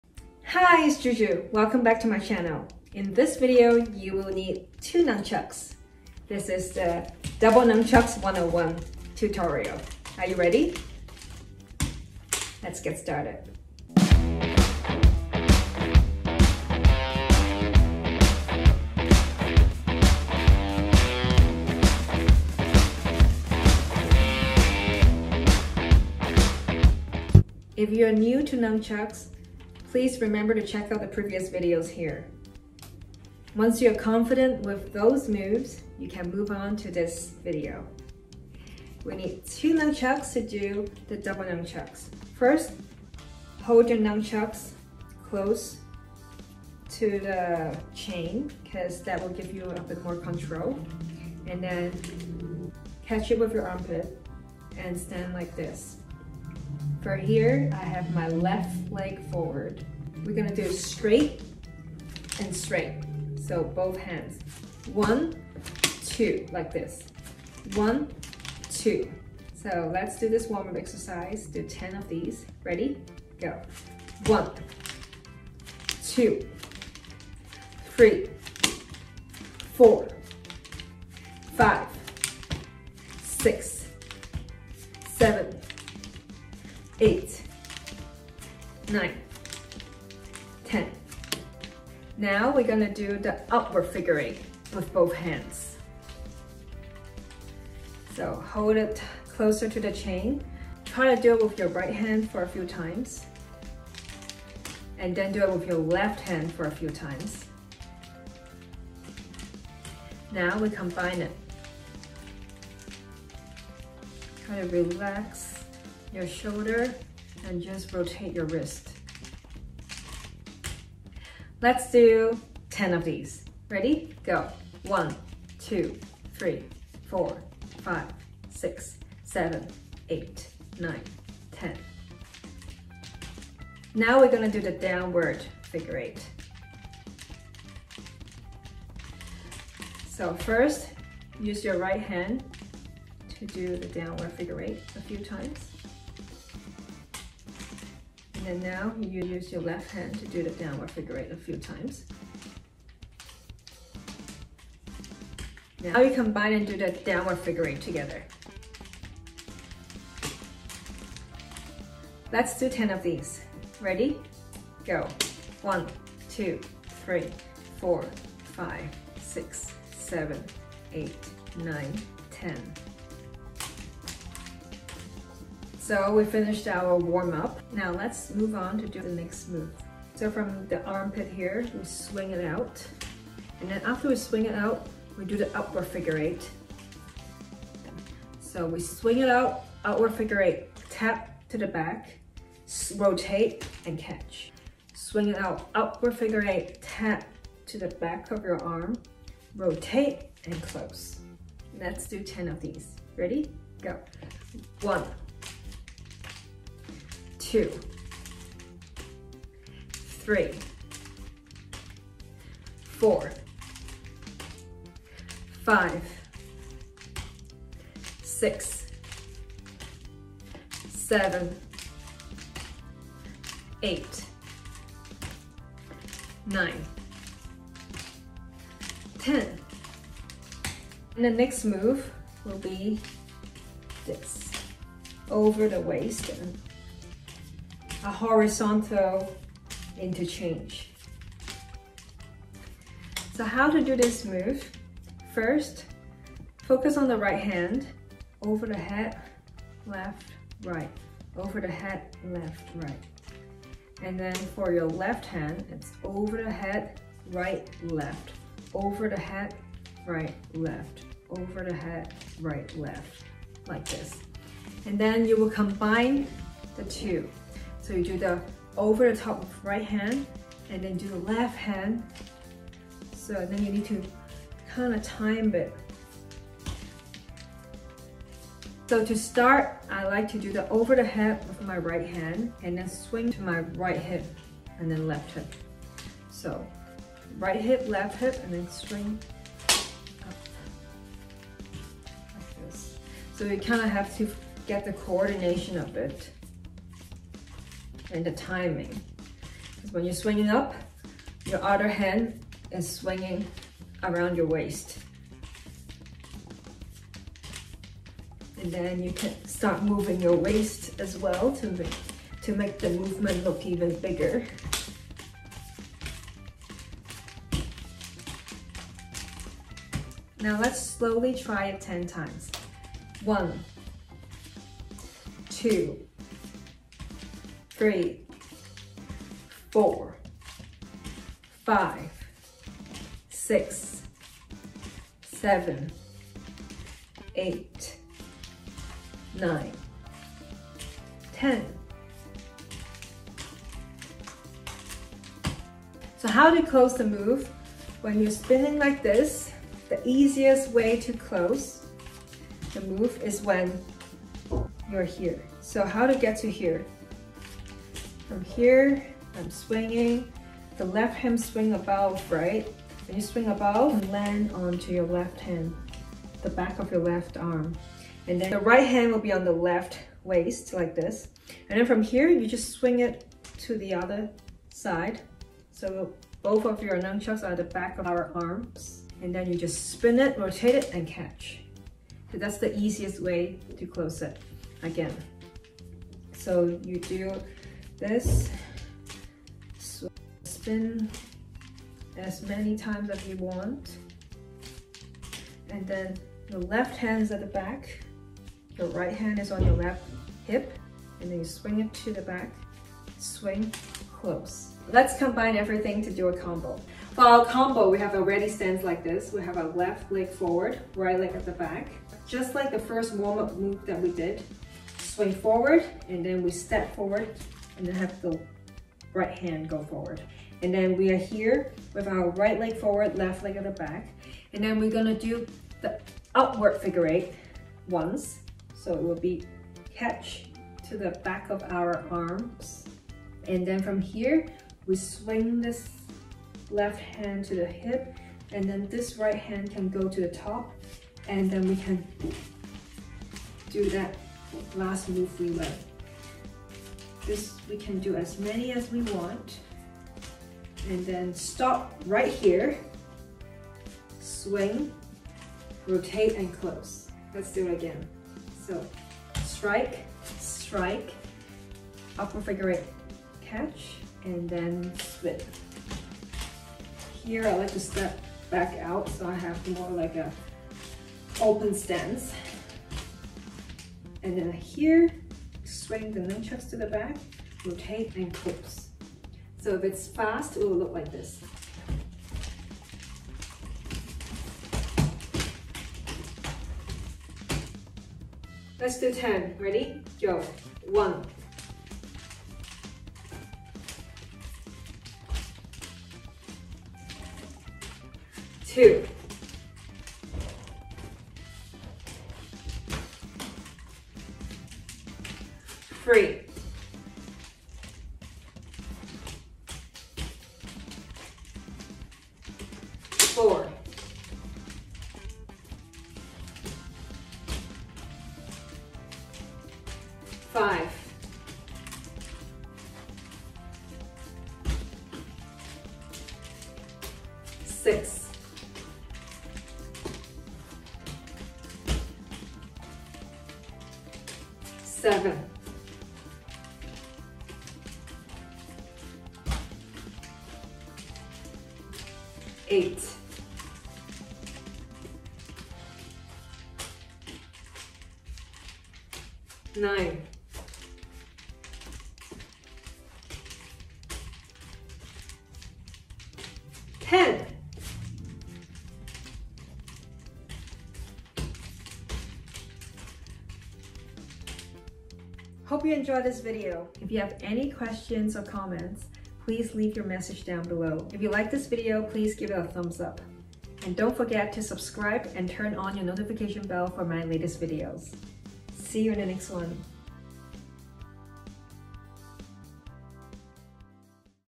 How to use nunchucks #8 : 10 mins Double Nunchucks FOLLOW ALONG (Beginner’s Level)
In this 10 mins nunchucks follow along video, you will learn the basics of double nunchucks techniques and a combo.

In this video I am wearing : Sweaty Betty